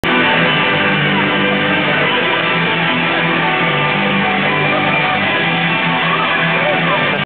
All the girls in the family dancing at the Bolingbrook Promenade, Thanksgiving 2007